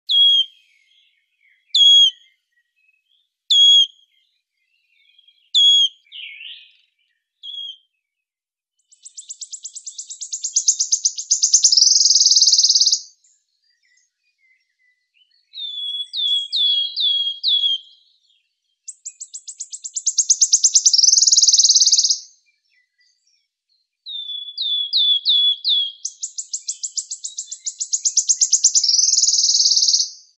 Audio del Mosquitero silbador.HD
Nombre científico: Phylloscopus trochilus

El mosquitero musical, un ave cuyo nombre científico es Phylloscopus trochilus, es un Passeriforme de la familia Phylloscopidae muy común y propio de las áreas templadas de Europa y Asia